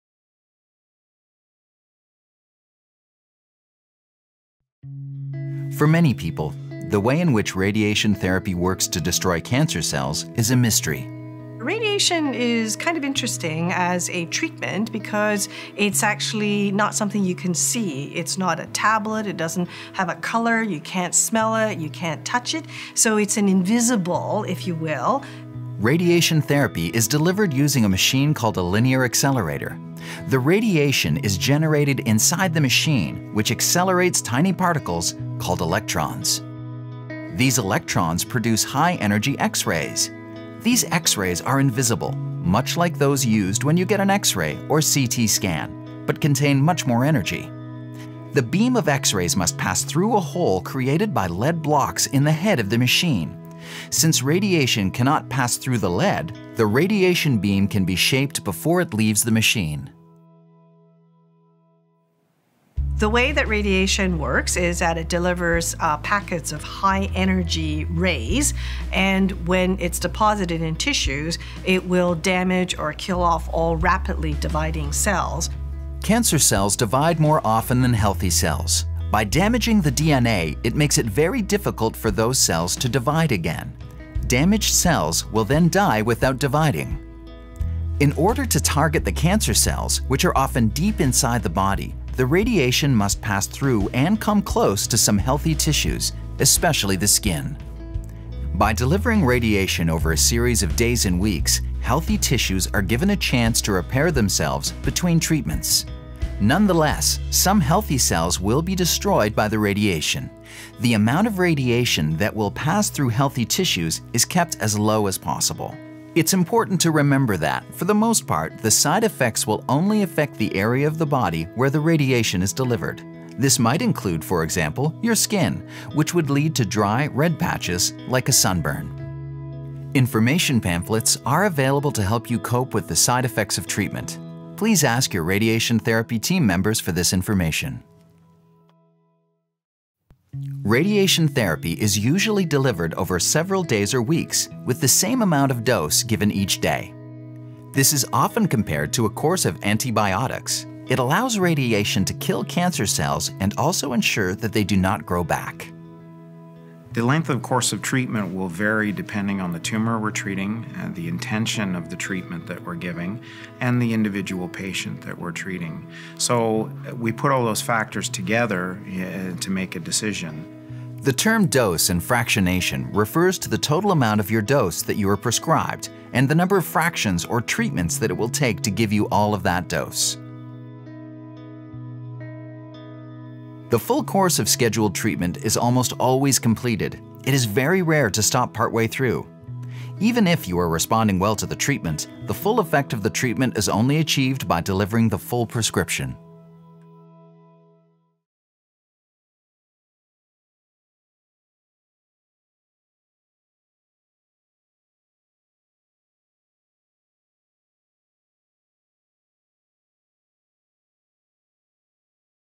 What to Expect: Radiation Therapy 101 [Part 7 of 7]
What to Expect: Radiation Therapy
A Patient Education Video Series: 7 of 7

Radiation Therapy 101
Common questions: How does radiation work?  How does it kill the cancer cells but not the healthy cells? This module provides a short but useful overview of how radiation is used in the treatment of cancer.

For more information on the Accelerated Education Program, visit us